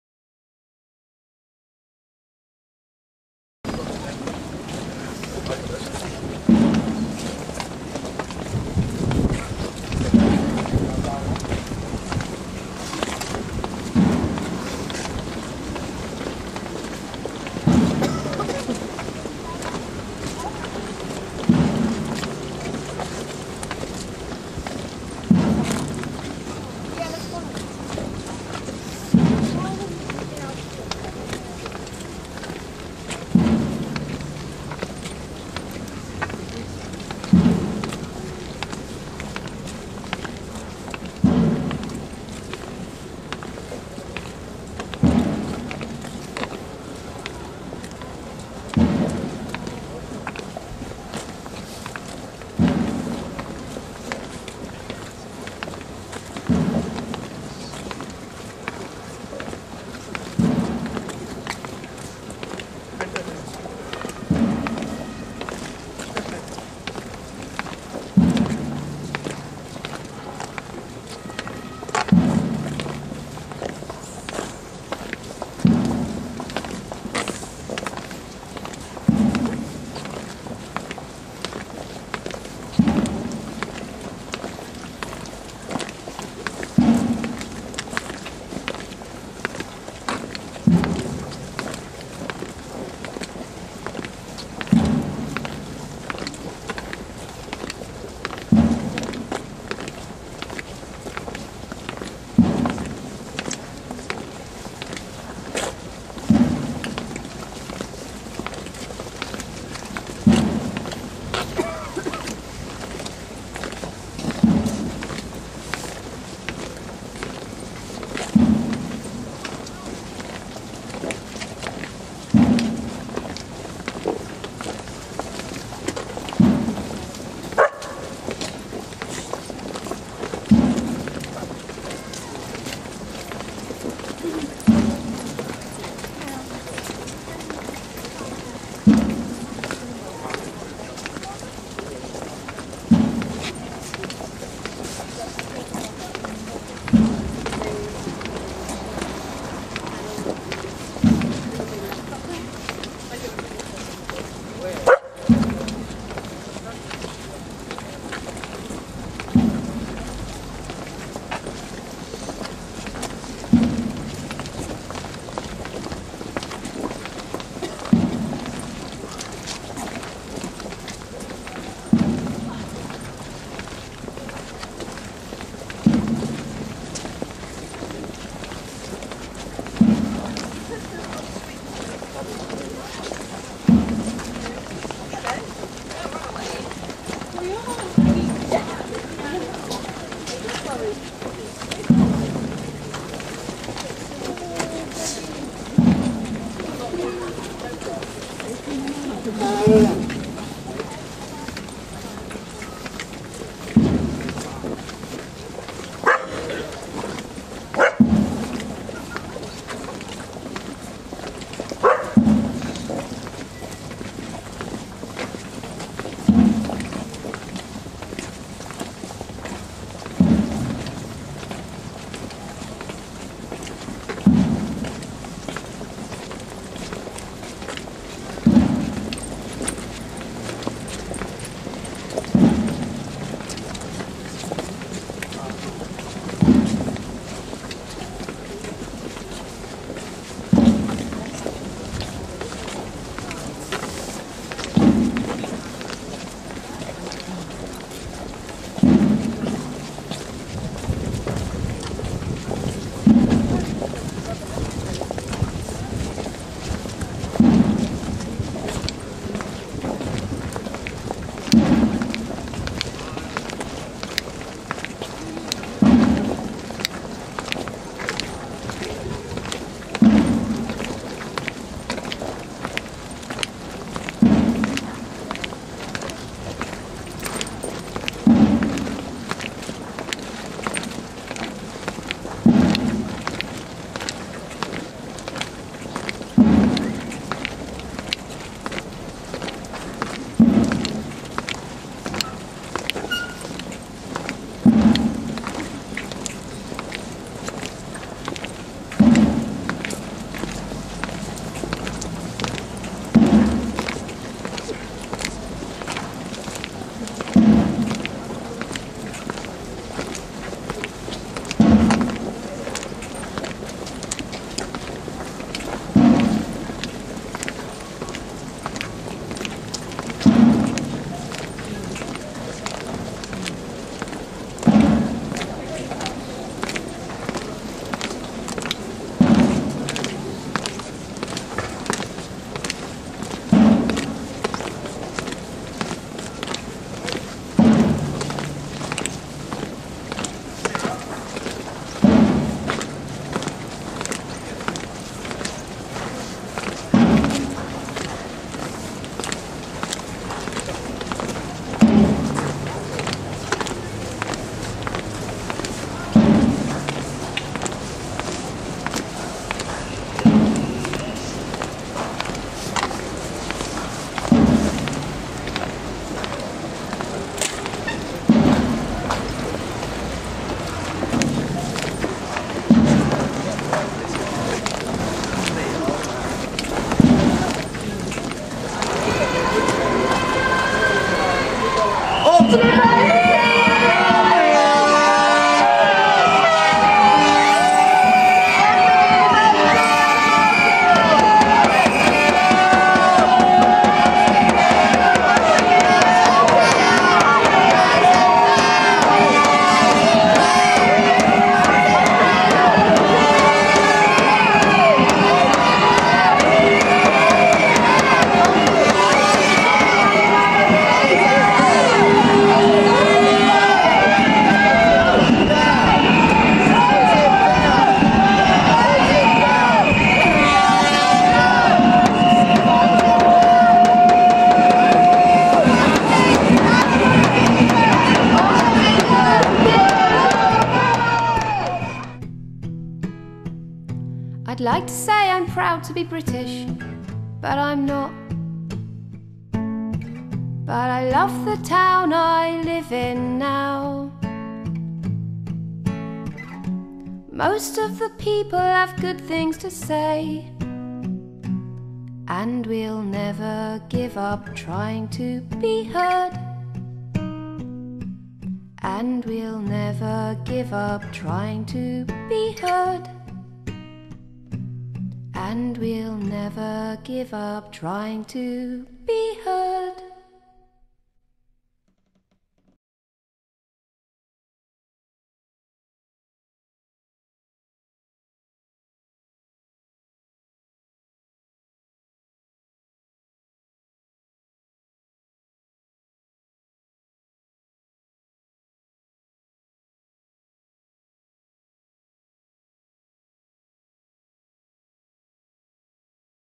The Speak Campaign & Oxford University Shame part 6 of 6
In the last part of The SPEAK Campaign & Oxford University Shame, Speak protesters continue their silent procession through the heart of Oxford.  As previously, (see part five) this is accompanied by an eclectic list of well known people who have been against animal experimentation.  Finally, the silence of the procession is broken and we are sung out, as we were in, by Tracey Curtis.